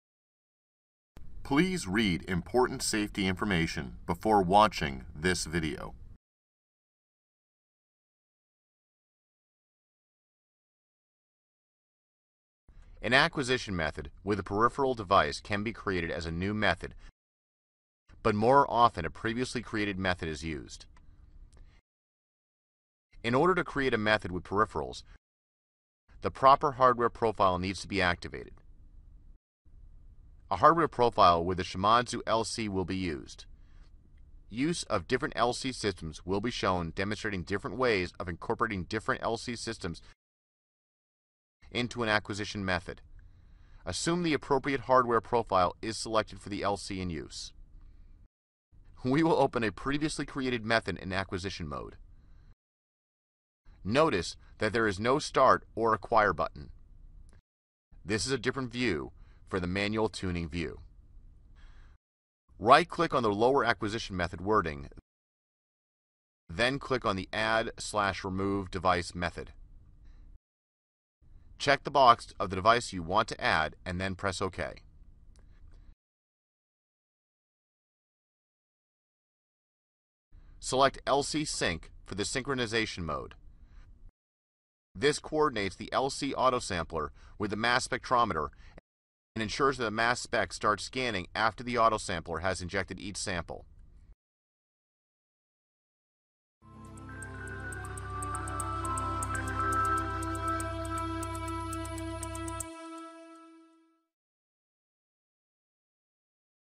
Create Acquisition Method with Peripherals in Analyst® Software
This video demonstrates how to create an LC/MS acquisition method with peripherals in Analyst® Software.  For more training please visit our learning center